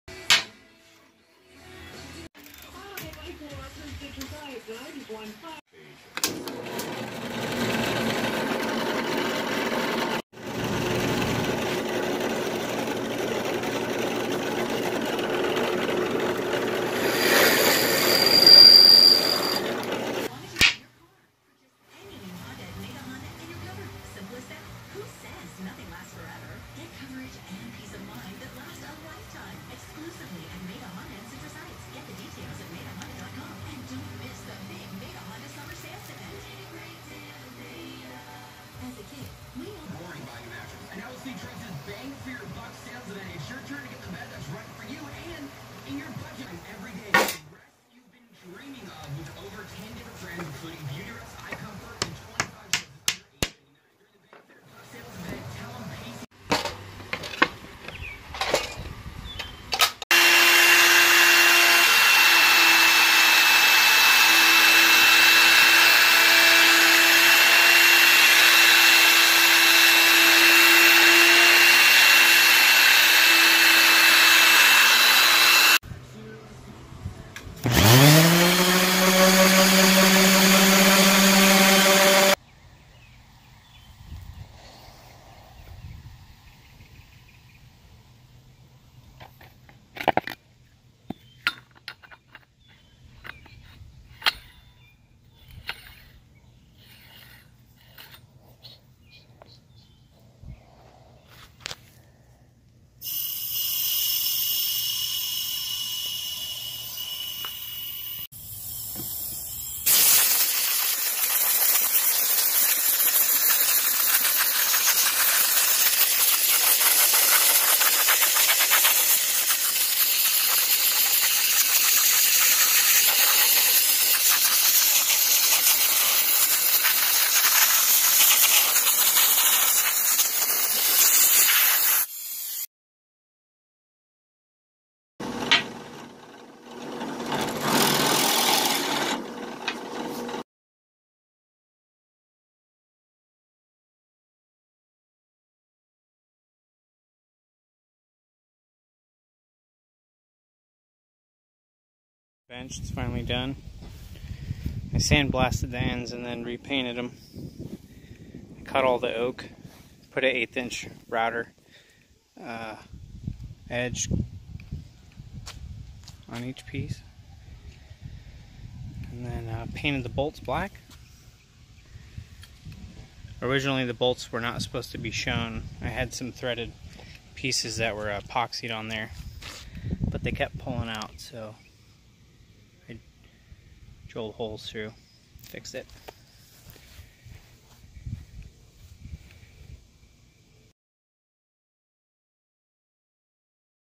Making a Wood Bench
I bought some old metal bench sides a year ago with now wood. This video is a quick highlight of the process entailed to restore and create a new bench.